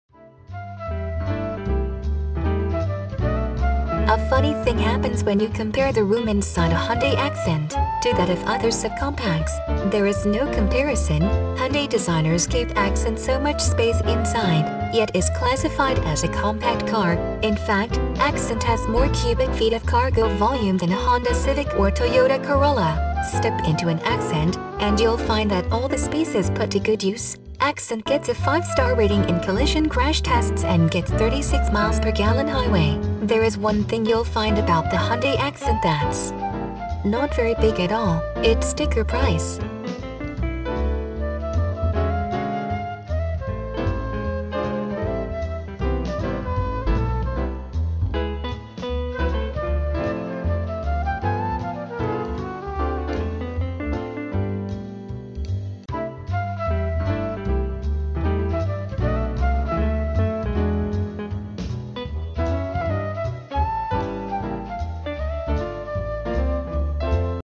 2012 HYUNDAI Accent ACCENT: 4DR SDN AUTO GLS
A funny thing happens when you compare the room inside a Hyundai Accent, to that of other sub-compacts. There is no comparison. Hyundai designers gave Accent so much space inside, yet is classified as a Compact Car. In fact, Accent has more cubic feet of cargo volume than a Honda Civic or Toyota Corolla. Step into an Accent, and you'll find that all the space is put to good use. Accent gets a "Five Star" rating in collision crash tests and gets 36 mpg highway. There is one thing you'll find about the Hyundai Accent that's not very big at all: It's Sticker Price.